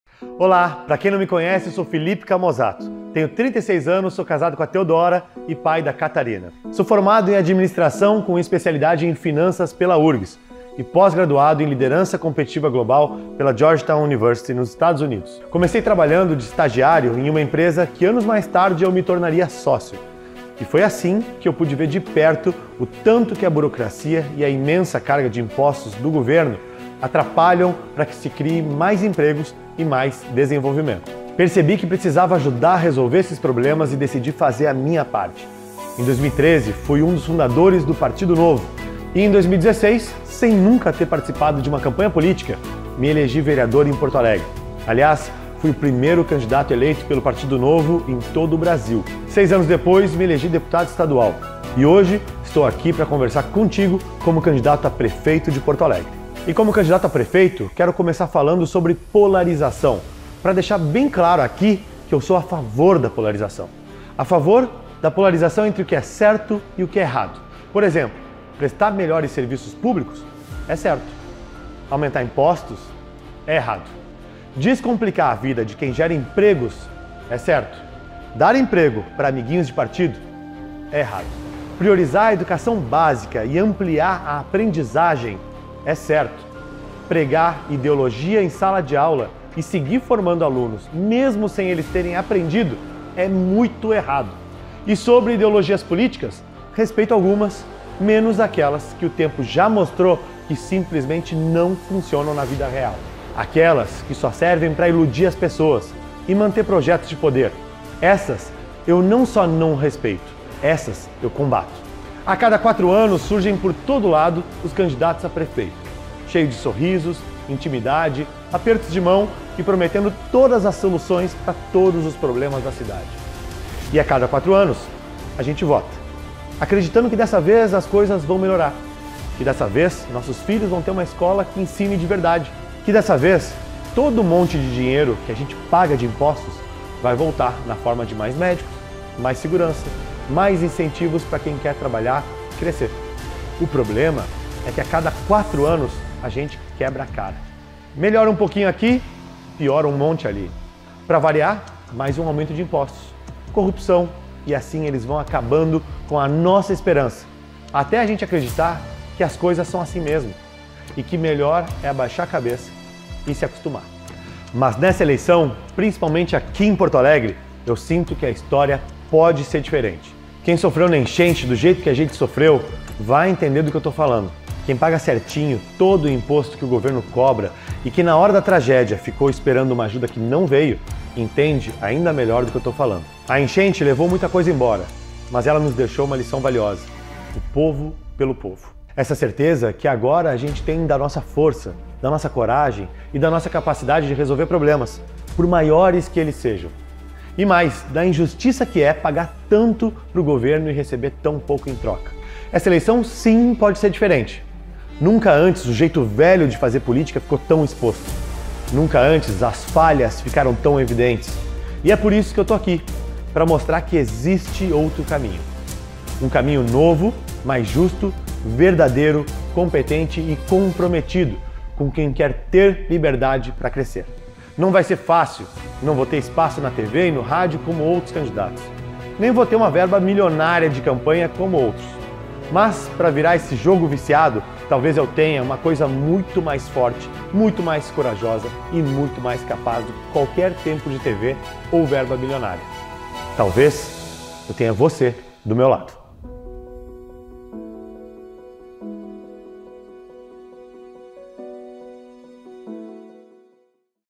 Prazer, sou o Felipe Camozzato! Candidato a prefeito de Porto Alegre.
Neste vídeo te convido a me conhecer um pouco mais!